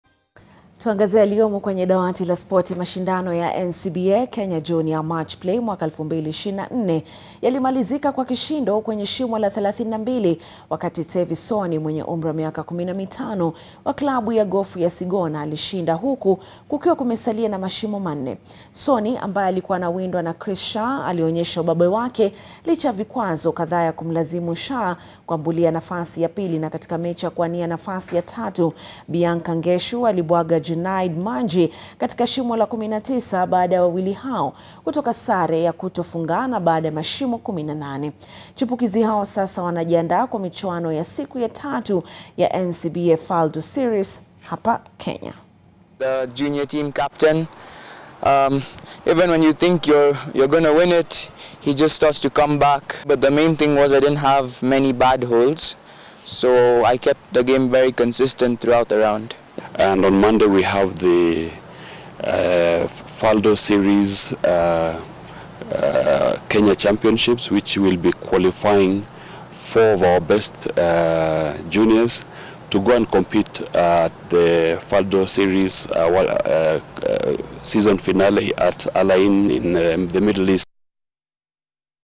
Tsevi Soni ashinda katika mashindano ya NCBA Kenya Junior Matchplay akisalia mashimo manne
Mashindano ya NCBA Kenya Junior Matchplay MWAKA 2024 yalimalizika kwa kishindo kwenye shimo la 32 wakati Tsevi Soni mwenye umri wa miaka 15 wa Klabu ya Gofu ya Sigona alishinda huku kukiwa kumesalia na mashimo manne.